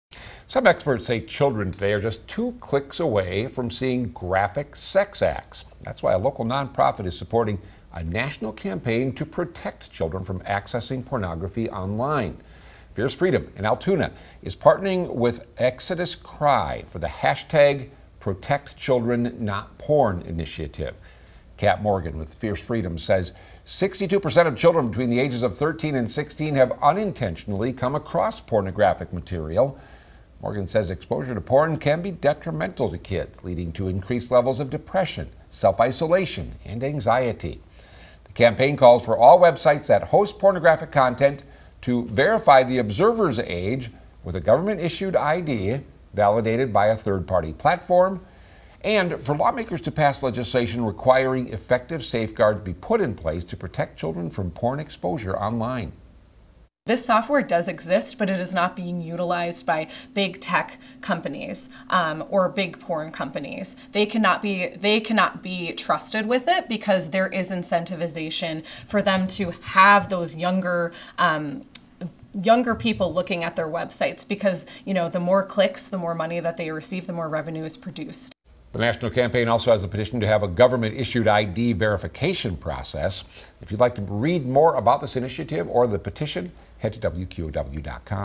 Fierce Freedom promotes Protect Children Not Porn campaign
Some experts say children today are just two clicks away from seeing graphic sex acts.
That's why a local nonprofit is supporting a national campaign to protect children from accessing pornography online.